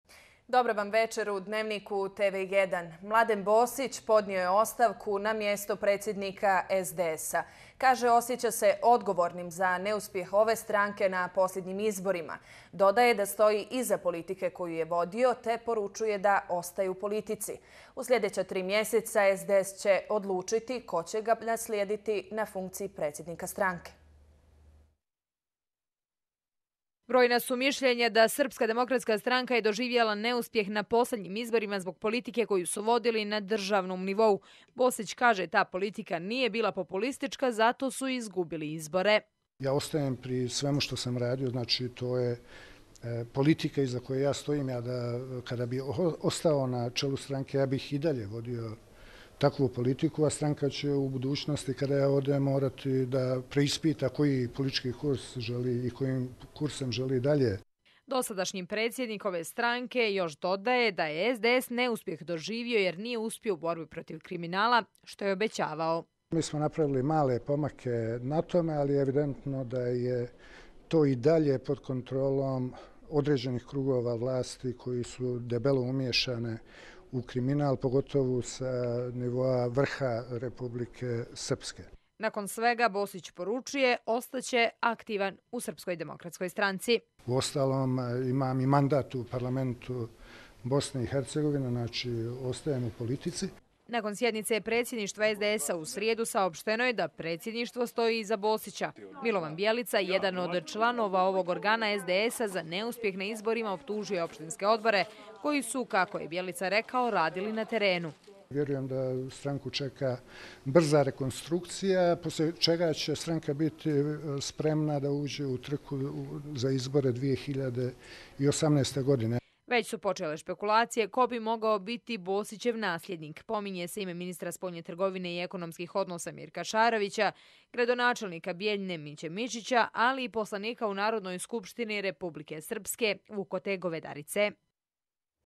Mladen Bosić podnio ostavku na mjesto predsjednika SDS
Mladen Bosić podnio je ostavku na mjesto predsjednika SDS-a. Kaže osjeća se odgovornim za neuspjeh ove stranke na posljednjim izborima. Dodaje da stoji iza politike koju je vodio, te poručuje da ostaje u politici. U  sljedeća tri mjeseca SDS će odlučiti ko će ga naslijediti na funkciji predsjednika stranke. Brojna su mišljenja da je SDS doživio neuspjeh na poslednjim izborima zbog politike koju su vodili na državnom nivou. Bosić kaže ta politika nije bila populistička zato su i izgubili izbore.

„Ja ostajem pri svemu što sam radio, to je politika iza koje ja stojim, kada bih ostao na čelu stranke ja bih takvu politiku, a stranka nakon što ja odem treba da preispita kojim kursem želi dalje“, kaže Mladen Bosić.

Bosić još dodaje, SDS je neuspjeh doživio jer nije uspio u borbi protiv kriminala što je obećavao: „Mi smo napravili male pomake na tome, ali je evidentno da je to i dalje pod kontrolom određenih krugova vlasti koje su debelo umješane u kriminal pogotovo sa nivoa vrha RS.“

Nakon svega dosadašnji predsjednik SDS-a ostaje u aktivan u stranci. „Imam i mandat u Parlamentu BiH, dakle ostajem u politici“, potcrtava Bosić.

Nakon sjednice predsjedništva SDS, u srijedu, saopšteno je da predsjedništvo stoji iza Bosića. Milovan Bjelica, jedan o članova ovog organa SDS-a za neuspjeh na izborima optužio je opštinske odbore, koji su kako je Bjelica rekao radili na terenu.

„Vjerujem da stranku čeka brza rekonstrukcija poslije čega stranka čeka da udje u trku za izbore 2018.godine“, kazao je dosadašnji predsjednik SDS.

Već su počele špekulacije ko bi mogao biti Bosićev naslijednik, pominje se ime ministra spoljne trgovine i ekonomskih odnosa Mirka Sarovica, gradonačelnik Bijeljine Miće Mićića, ali i poslanika u NS RS Vukote Govedarice.